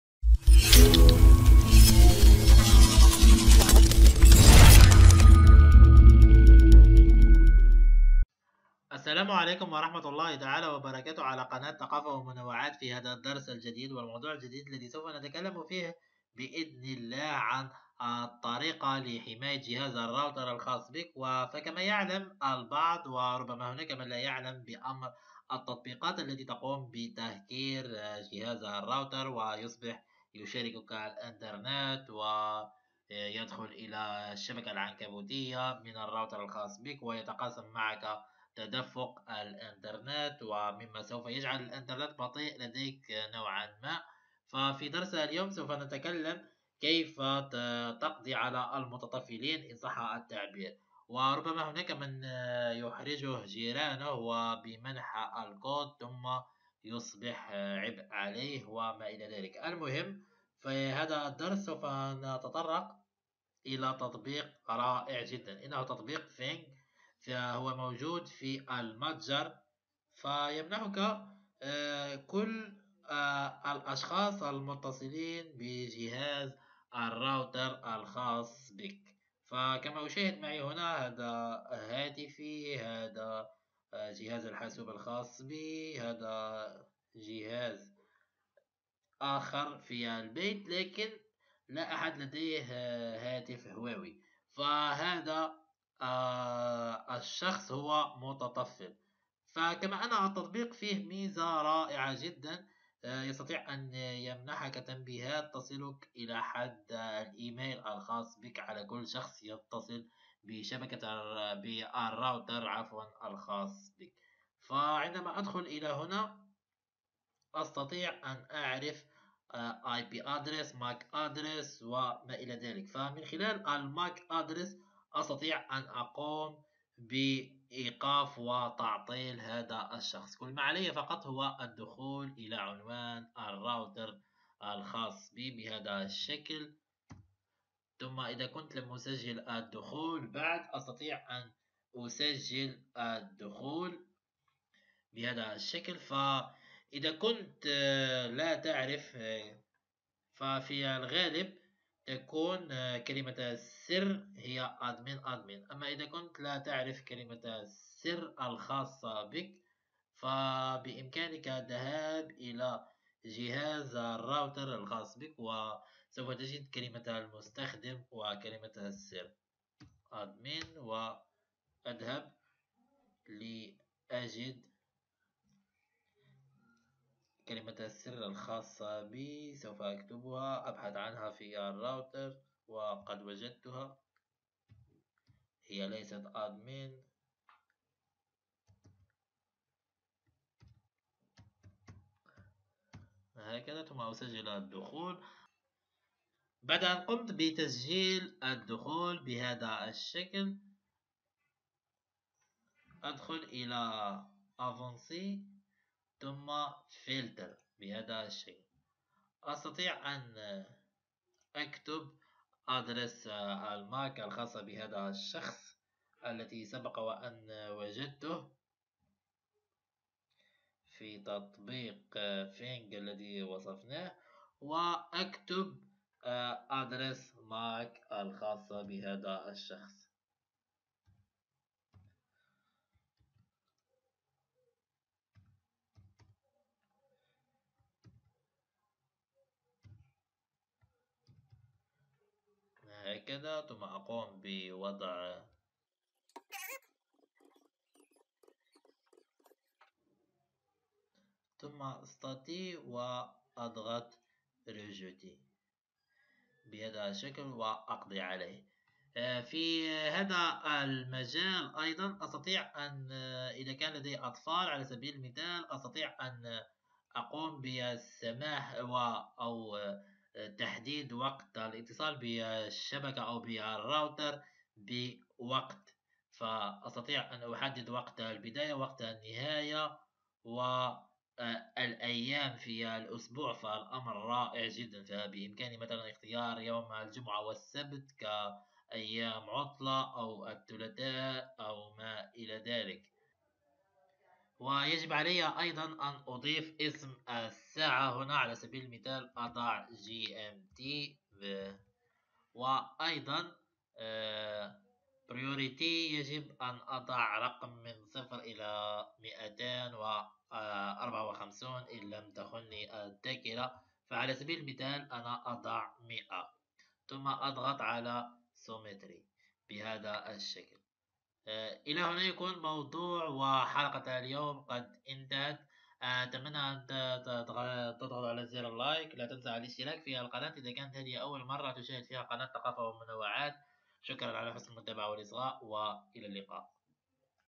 القضاء على المتطفيلين و تنظيف جهاز الراوتر و تسريع الأنترنت بشكل رهيب
في هذا الفيديو سوف نعرض عليك طريقة للتخلص من كل المتطفيلين الذين يضايقونك في جهاز الراوتر الخاص بك و يشاركونك تدفق الانترنت و يجعلون منها بطيئة كما تستمتع بتدفق الانترنت بشكل جميل جدا
سجل إعجابك بصفحتنا على الفيسبوك:
هل يخدعنا مشاهير حول العالم ، شاهدالفكرة و إحكم بنفسك؟سر خطير